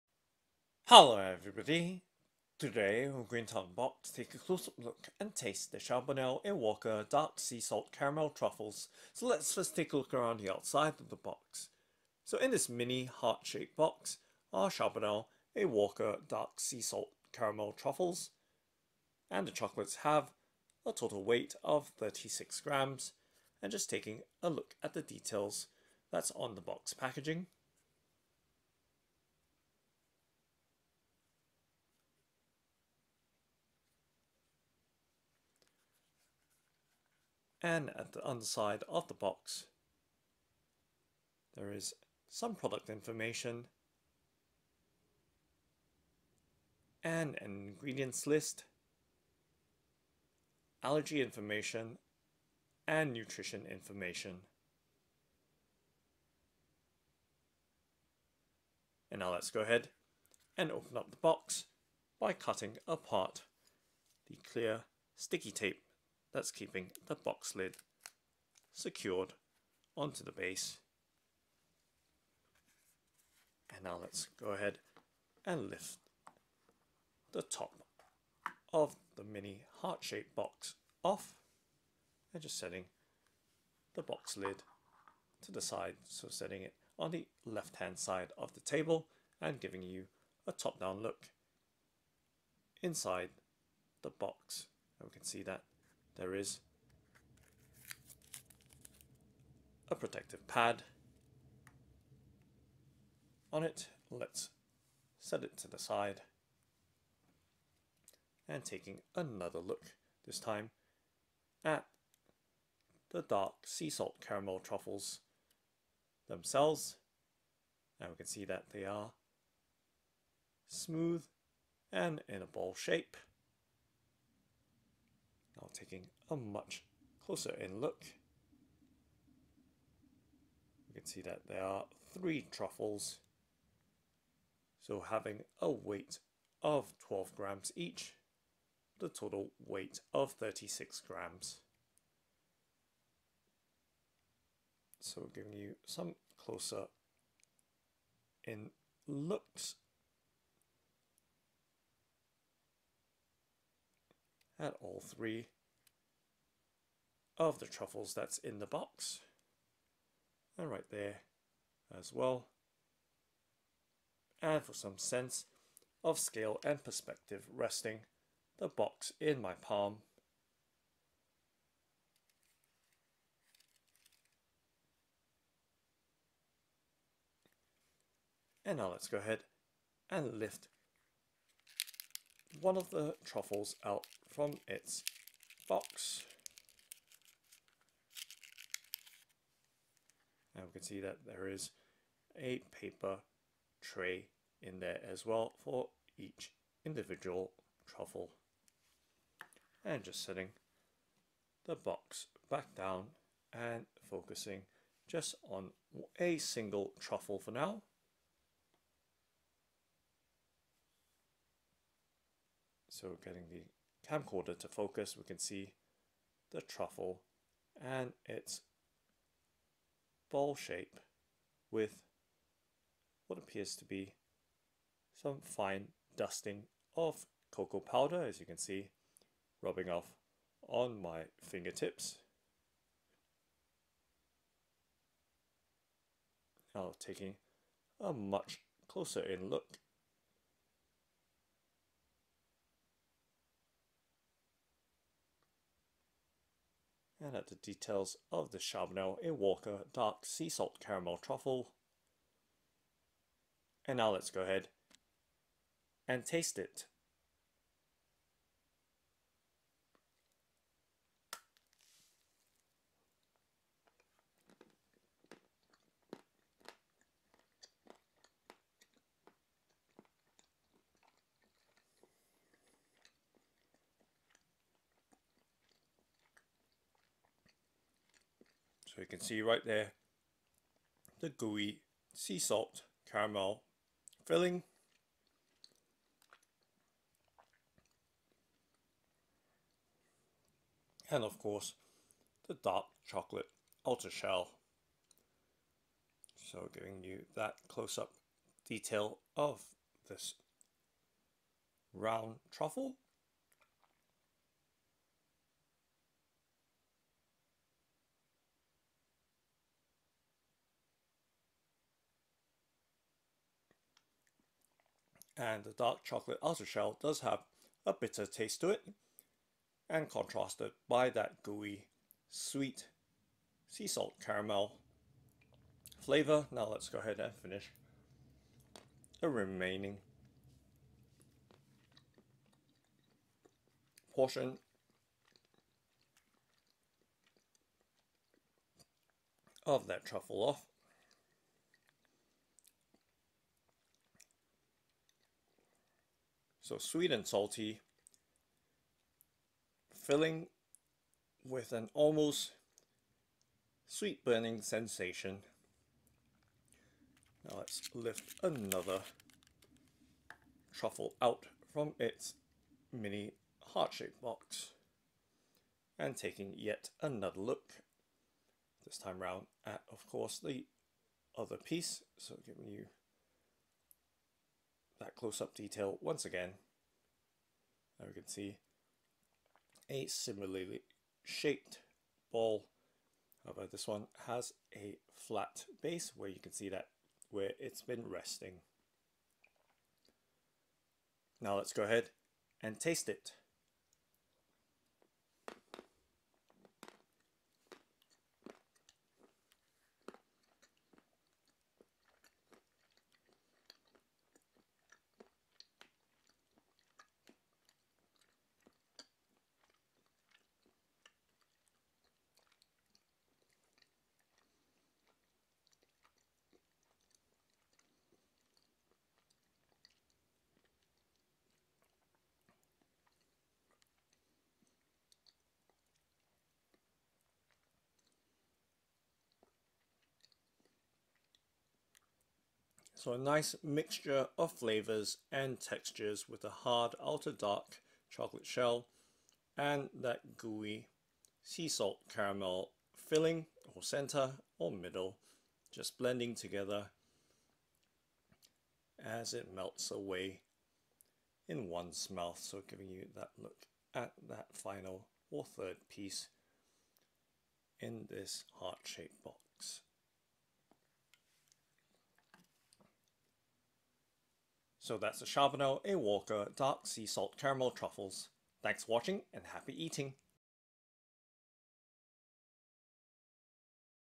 Charbonnel et Walker Dark Sea Salt Caramel Truffles - Blue Mini Heart
Unboxing the Charbonnel et Walker Dark Sea Salt Caramel Truffles - Blue Mini Heart. A closeup look and taste of the 36g box of dark chocolate balls with a gooey sea salt caramel filling. It is vegan friendly. The chocolate truffles come in a blue mini heart shaped box.

Dark Sea Salt Caramel Chocolate Truffles – Blue Mini Heart - Charbonnel et Walker UK

Chapters:
0:00 Product box packaging
0:36 Nutritional information and ingredients list
0:58 Opening the product box packaging
1:47 Charbonnel et Walker Dark Sea Salt Caramel Truffles
4:12 Tasting and closeup look of a half piece of Charbonnel et Walker Dark Sea Salt Caramel Truffle
5:25 Tasting another half piece of Charbonnel et Walker Dark Sea Salt Caramel Truffle
5:53 Charbonnel et Walker Dark Sea Salt Caramel Truffles
6:30 Tasting a Charbonnel et Walker Dark Sea Salt Caramel Truffle